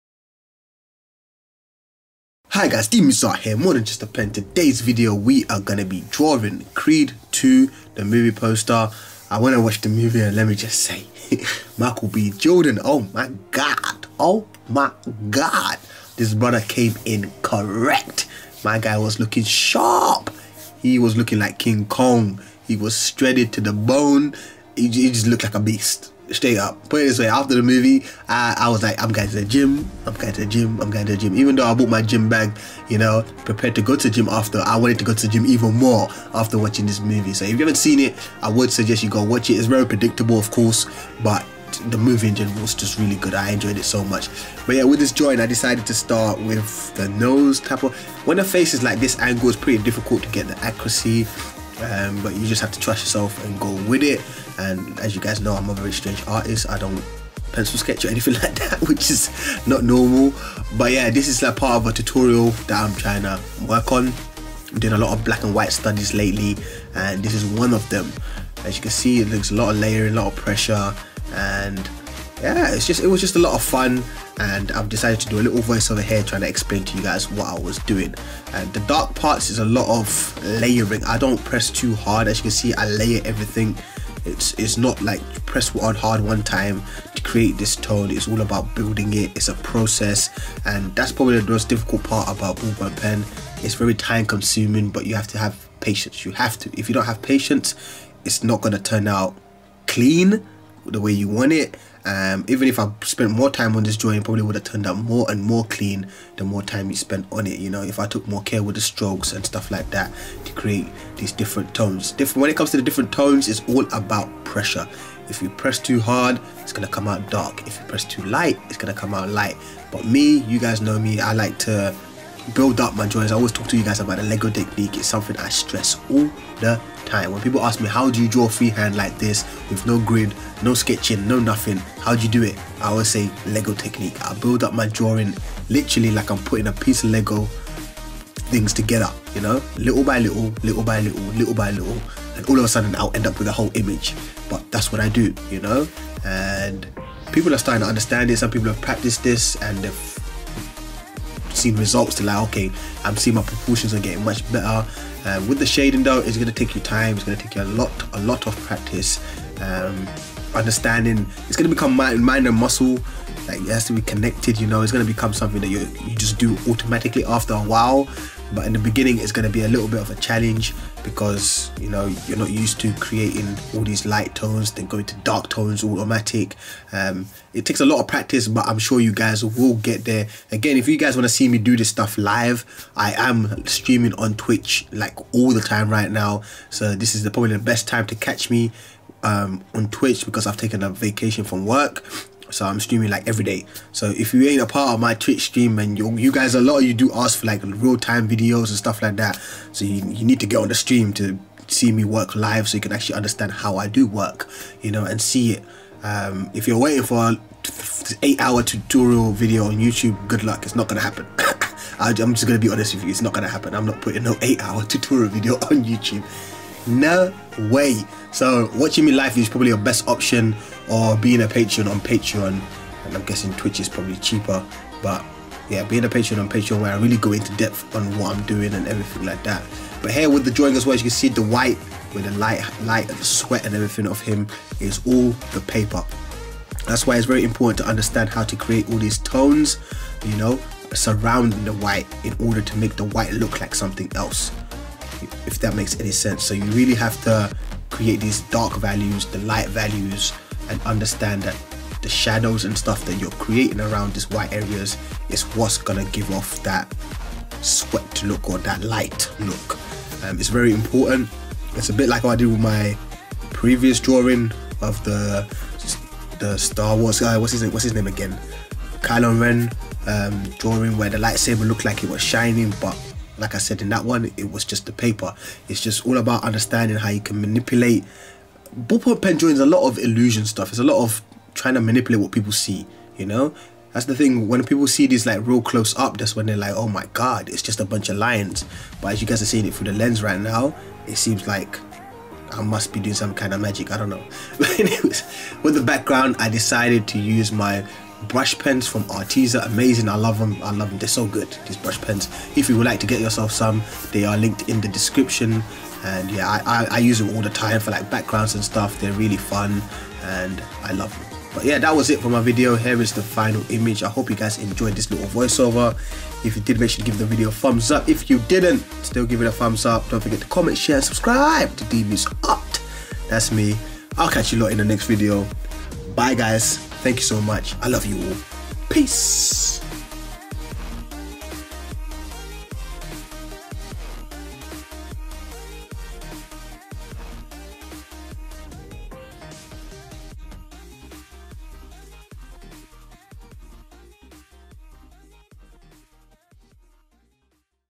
DRAWING MICHAEL B. JORDAN | CREED 2 - DeMoose Art
Michael B. Jordan ballpoint pen drawing. After watching the CREED 2 movie, I just had to do this drawing. Right now I am working on a lot of black and white pieces. In this video I will be talking to guys about layering and pressure control with the ballpoint pen.

Staedtler Ballpoint Stick Pens

BIC Soft Ball Point Pen

Watercolour 300gsm Paper

THIS DRAWING TOOK ME:
6 hours 35 minutes